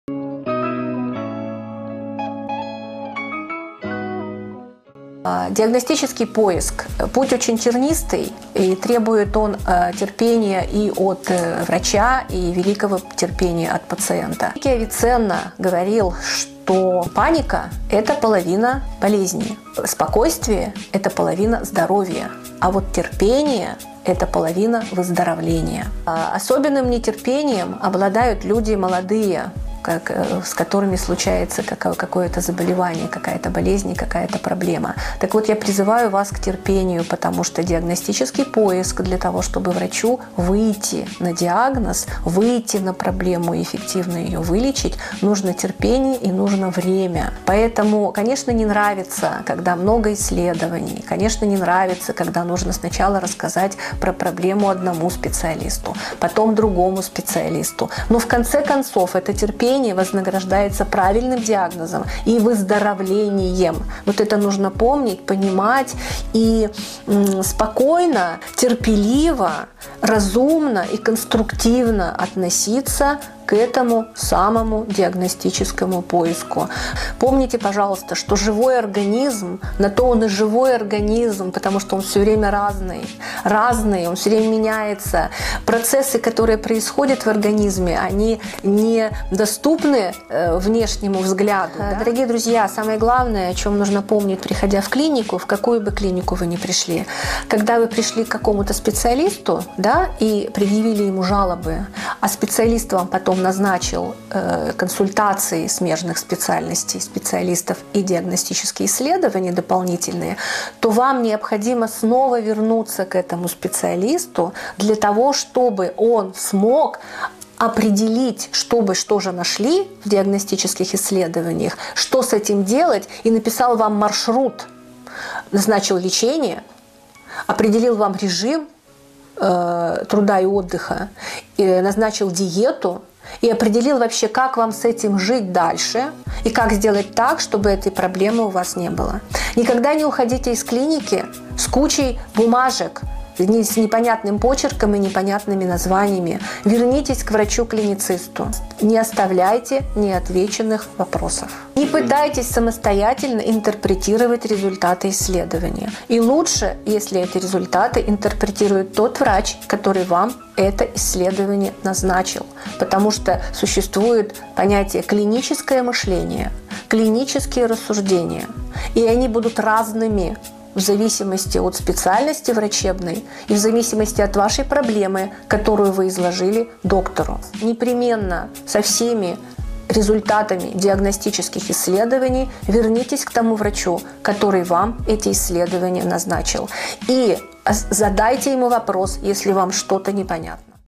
Процесс диагностического поиска. Наталья Афанасьевна Тубылова, главный врач клиники Медсервис
Великий Авиценна говорил, что паника - это половина болезни, спокойствие - это половина здоровья, а терпение - это половина выздоровления. Особенным нетерпением обладают молодые люди. Я призываю вас к терпению. Потому что диагностический поиск требует терпения и времени. Конечно, не может нравиться, когда назначают диагностические исследования, когда о проблеме надо рассказать одному специалисту, потому другому. Но, в конце концов, терпение вознаграждается правильным диагнозом и выздоровлением.
Главное, что необходимо помнить, если вы пришли в клинику, к определенному врачу и он назначил вам дополнительные исследования и консультации врачей смежных специальностей, вам необходимо снова вернуться к этому специалисту, чтобы он смог написать вам маршрут лечения, определил режим труда и отдыха, диету и определил как вам жить дальше и сделать так, чтобы этой проблемы не было. Не уходите из клиники с кучей бумажек, исписанных непонятным почерком и названиями. Вернитесь к врачу-клиницисту, чтобы получить интерпретацию полученных результатов. Не оставляет вопросов без ответов.
Наталья Афанасьевна Тубылова, главный врач клиники Медсервис.